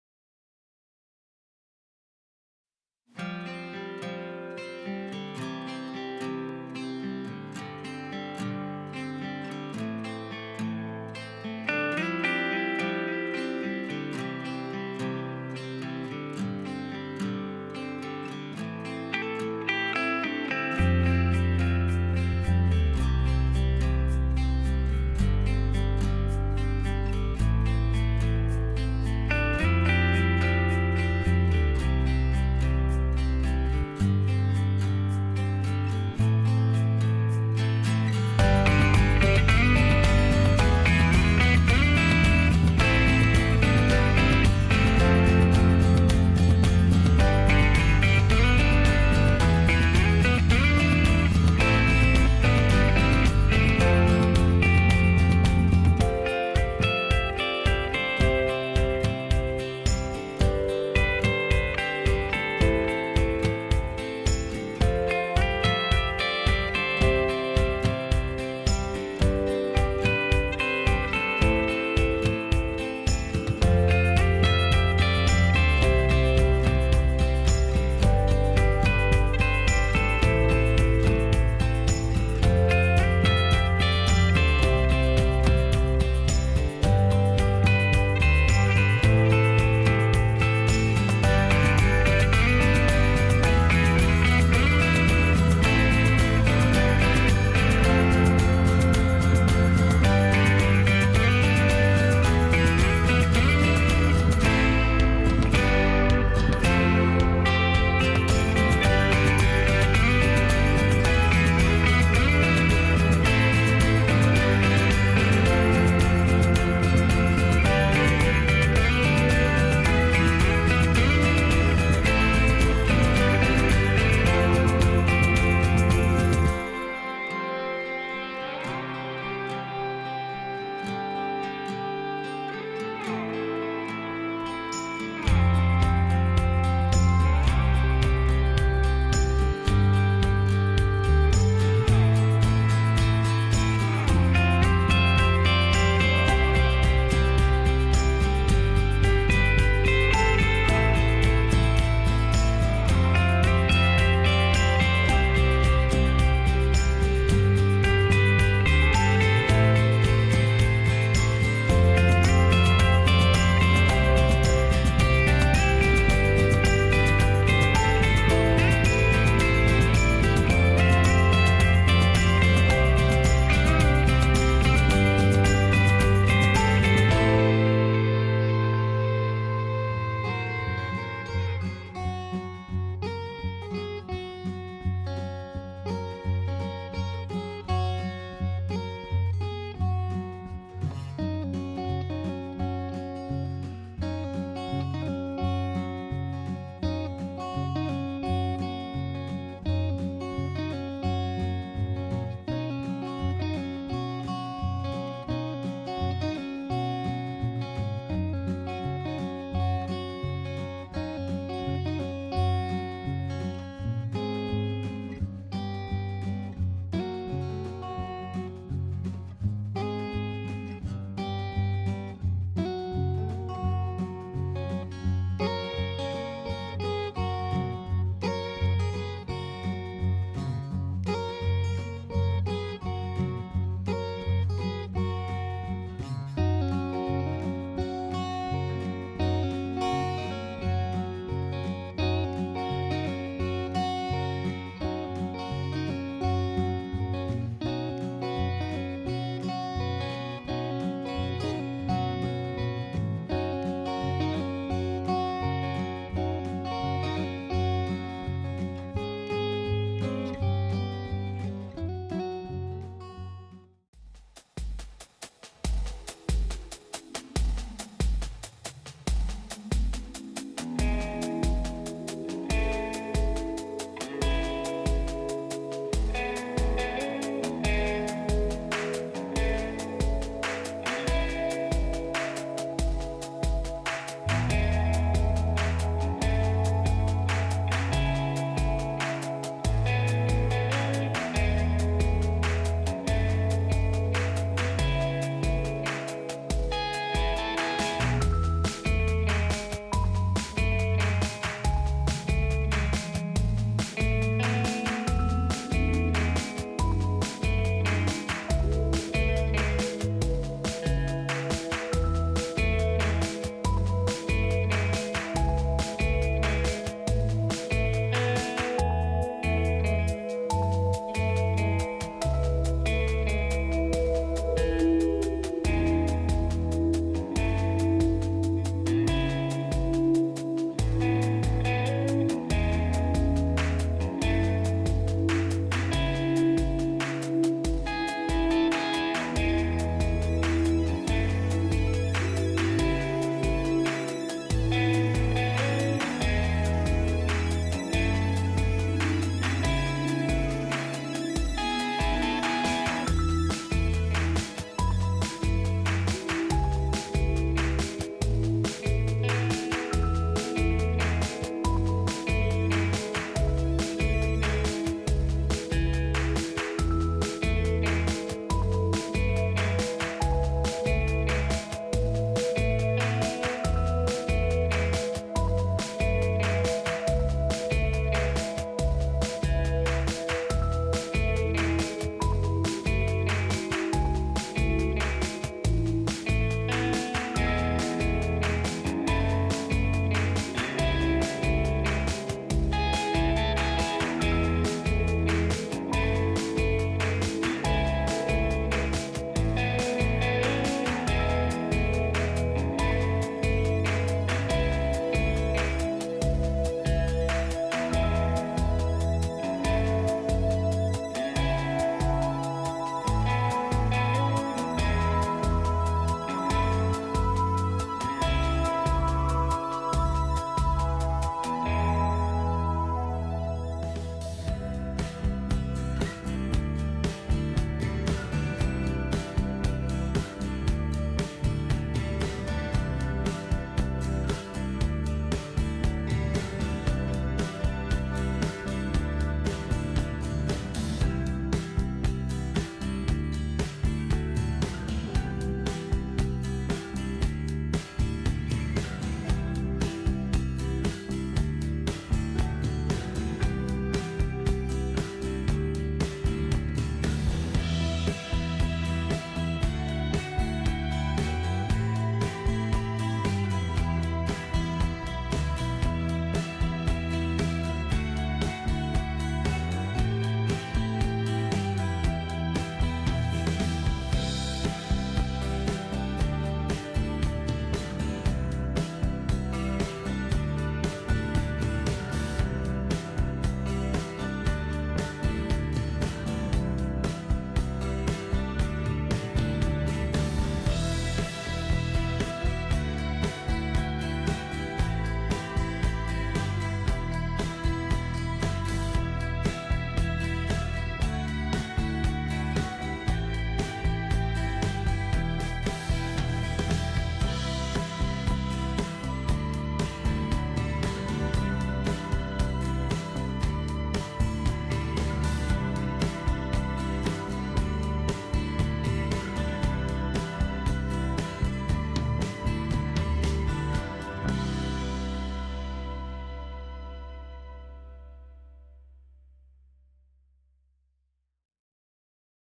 dji phantom 4 short films
Some shots from around Co Down from Ardglass to Nendrum and of course Northern Irelands tallest Light house at St Johns point.
the beauty of Northern Ireland is clear to see.